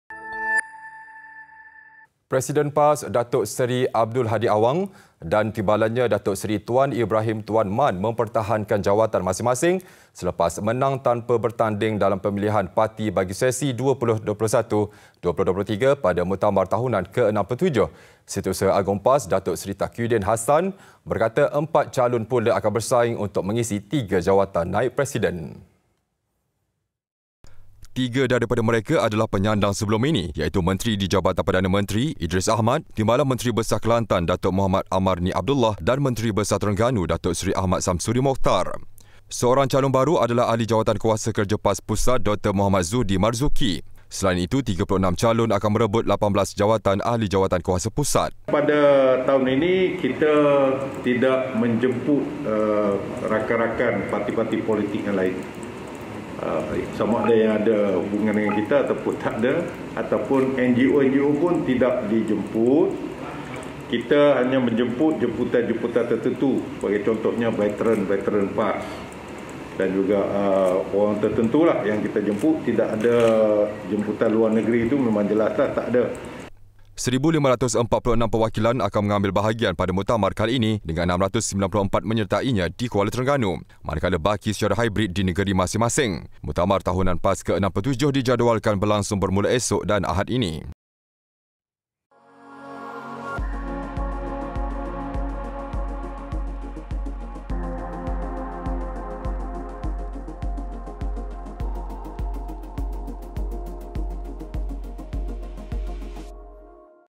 MUKTAMAR | Abdul Hadi Kekal Presiden PAS, Tuan Ibrahim Timbalan Presiden
Presiden PAS, Datuk Seri Abdul Hadi Awang, dan Timbalannya, Datuk Seri Tuan Ibrahim Tuan Man, mempertahankan jawatan masing-masing, selepas menang tanpa bertanding dalam pemilihan parti bagi sesi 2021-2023 pada Muktamar Tahunan ke-67.

Setiausaha Agung PAS, Datuk Seri Takiyuddin Hassan, berkata empat calon pula akan bersaing, untuk mengisi tiga jawatan naib presiden.